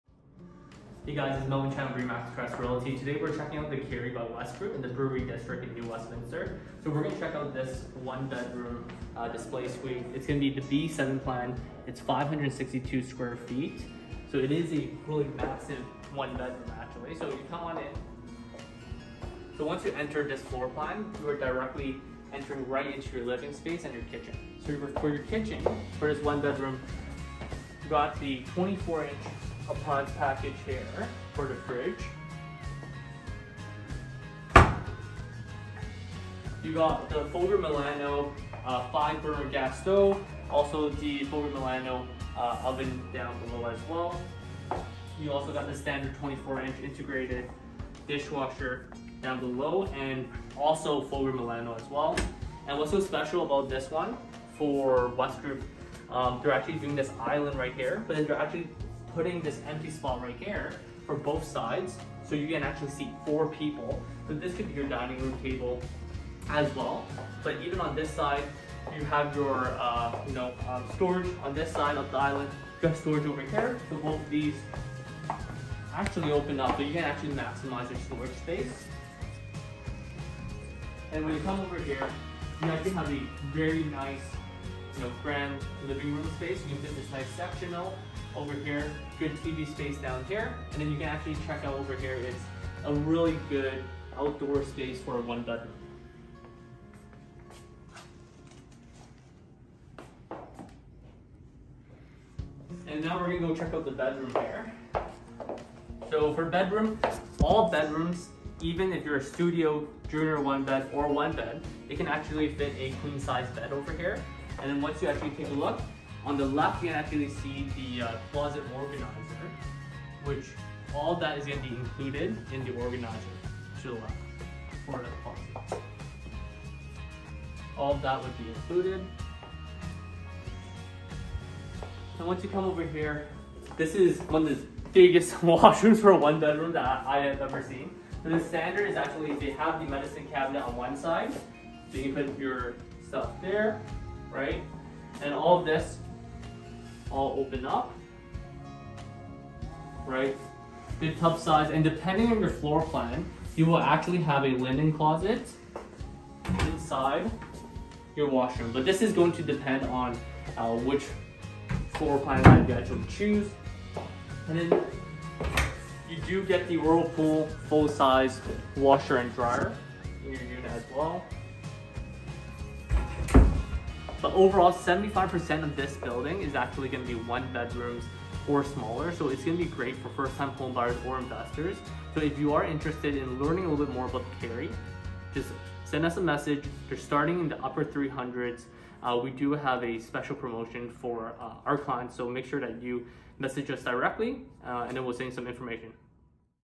The Keary by Wesgroup | New Westminster Presale | Brewery District
Starting in the upper $300,000s! The Keary by Wesgroup, the last pre-construction coming to the Brewery District by Wesgroup. Check out this video for a quick walkthrough of the show home. We are now booking private previews.